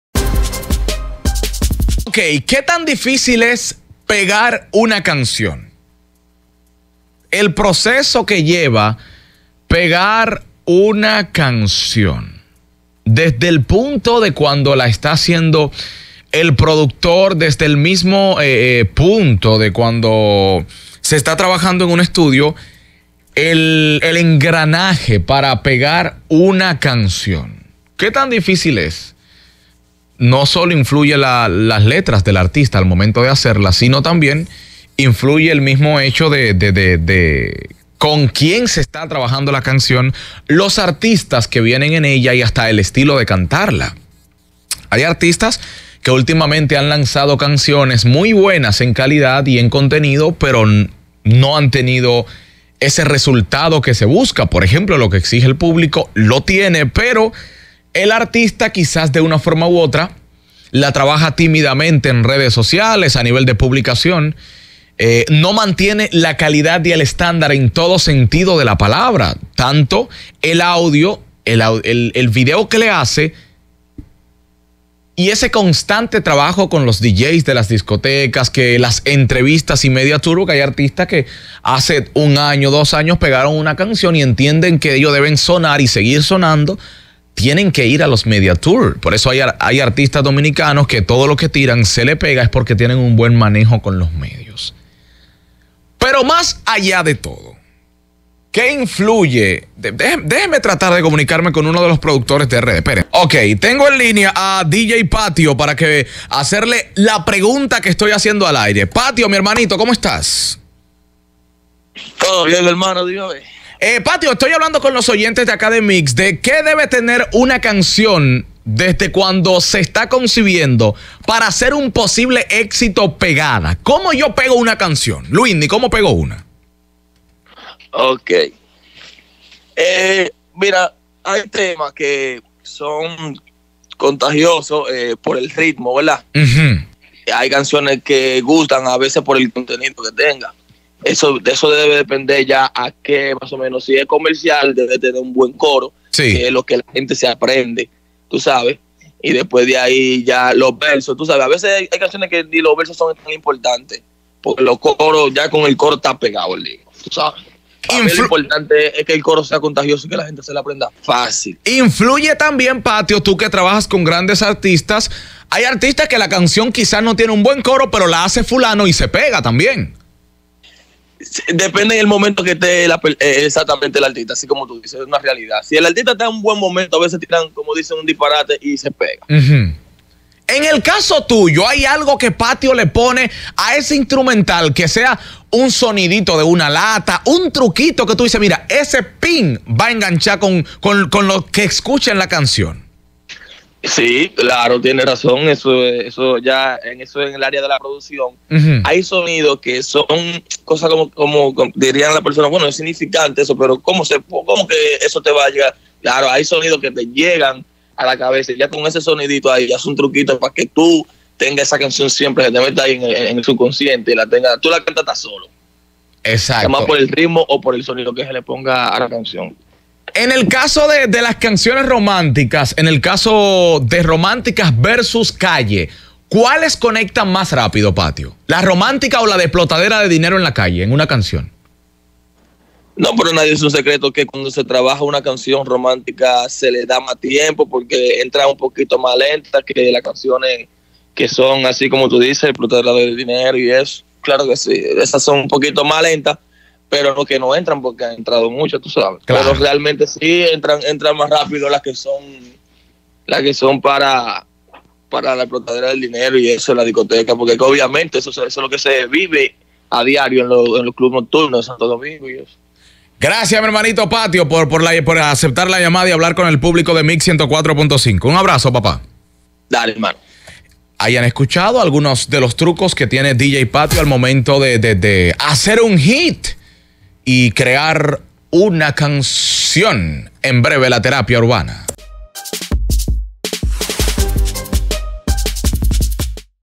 Dj Patio revela los detalles de como se crea un verdadero Hit Musical
Descarga la aplicación "Luinny Corporan'' para Android